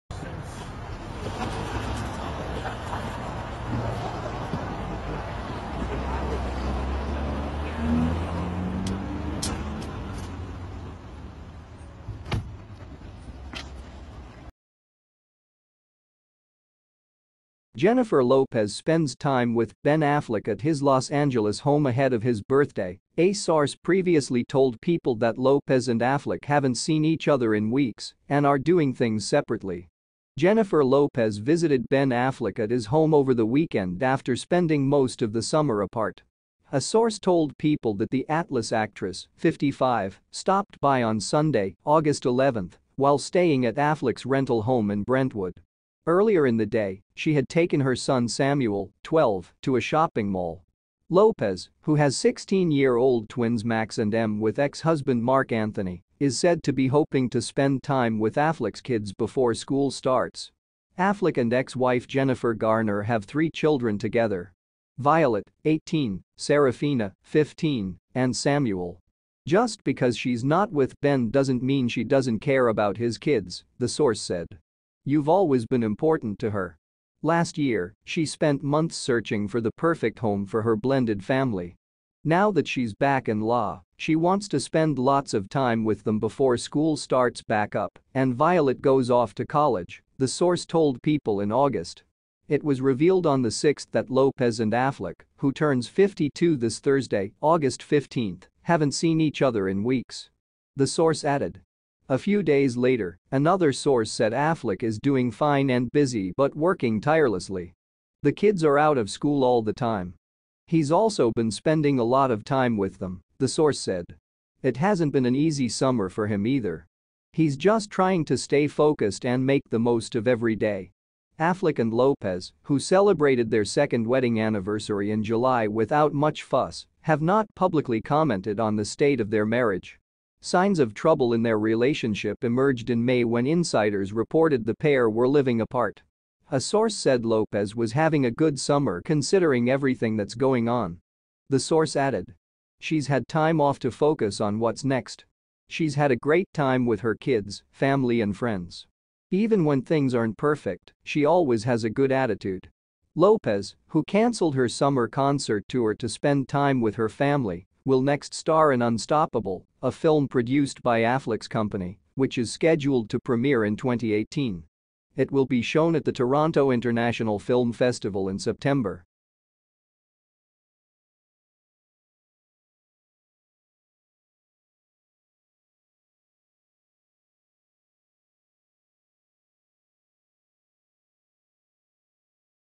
Jennifer Lopez Spends Time with Ben Affleck at His L.A. Home Ahead of His BirthdayJennifer
Jennifer Lopez Spends Time with Ben Affleck at His L.A. Home Ahead of His Birthday